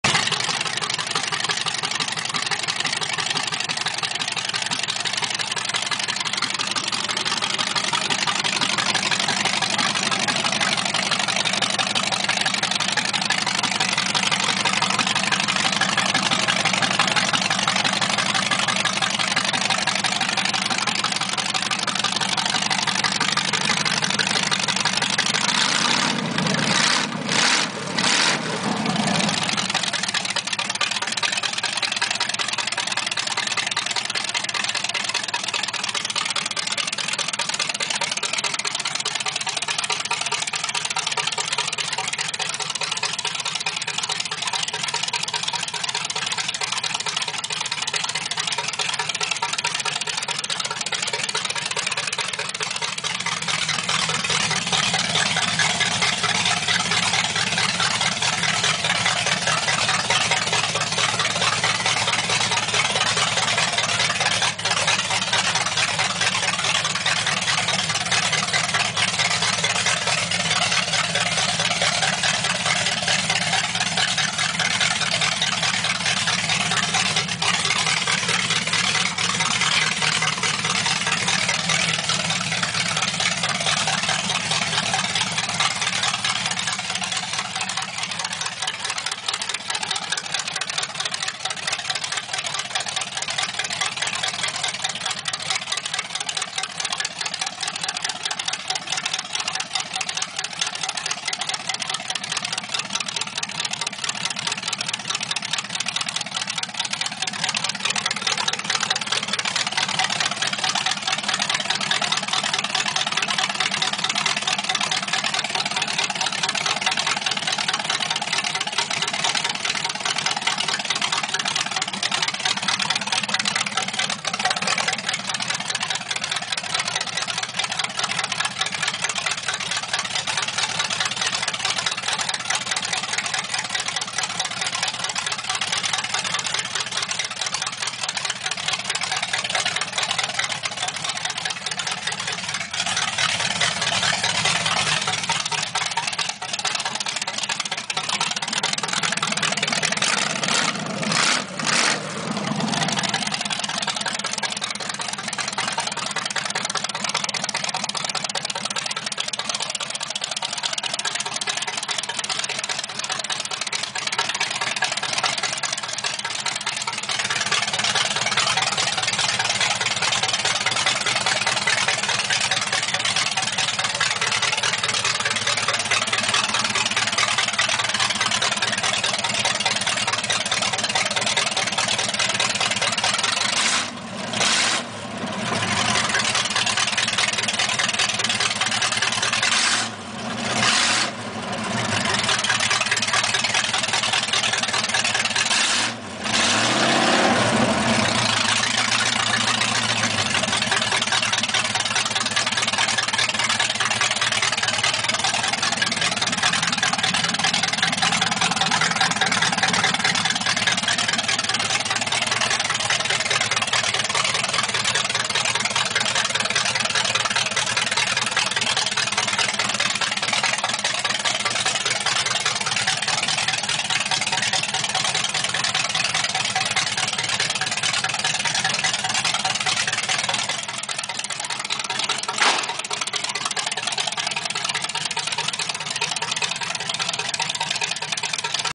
1930 Model A Pickup
1930 Model A Pickup -sold
TCI Frame
V8 283 SBC
TH350 Trans
8.5in Rearend